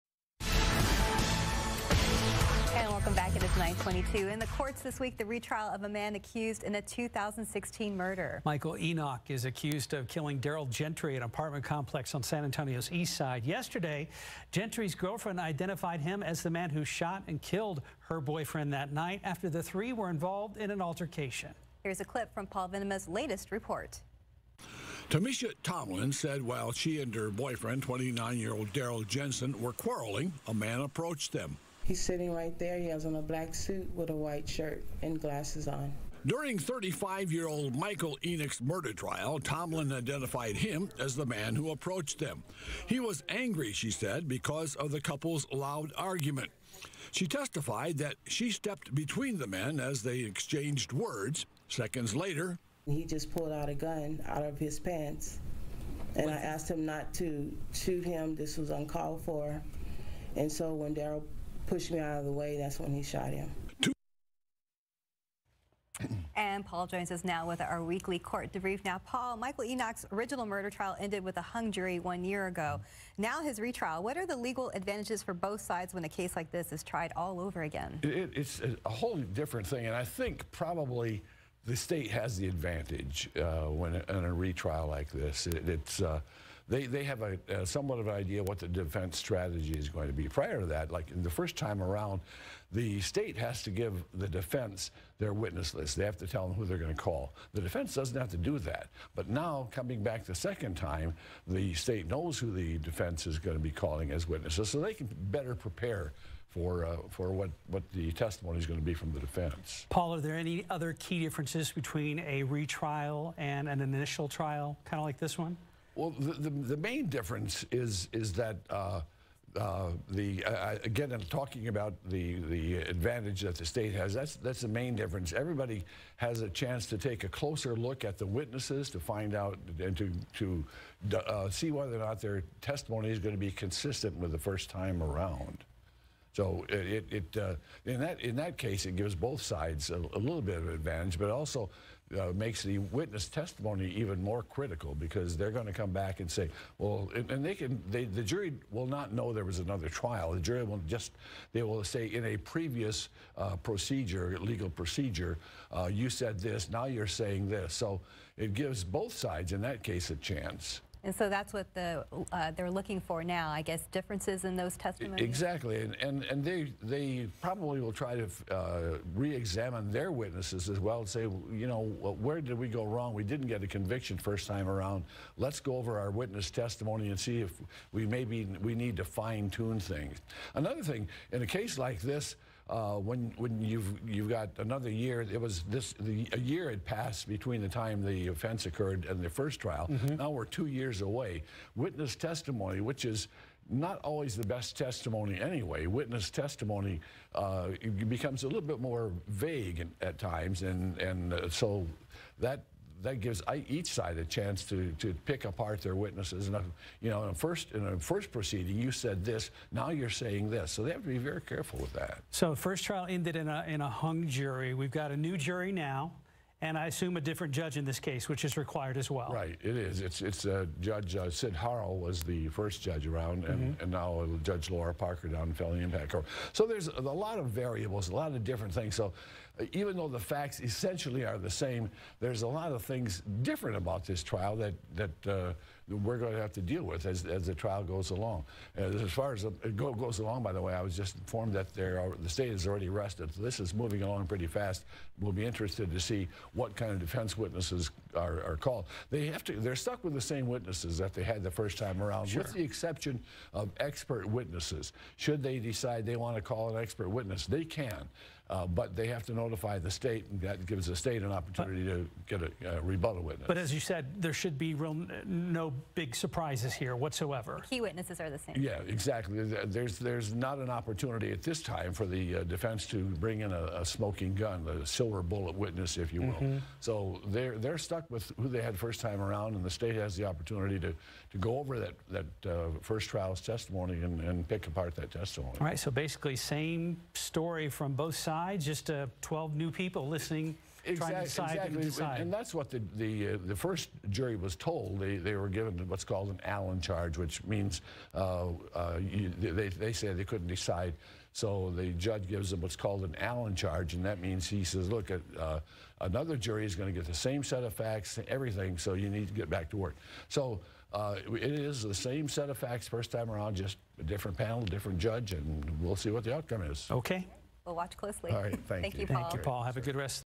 Court Debrief: Michael Enoch murder trial
KSAT 12 News reporter Paul Venema sits down with GMSA @ 9 to talk about the Michael Enoch murder trial.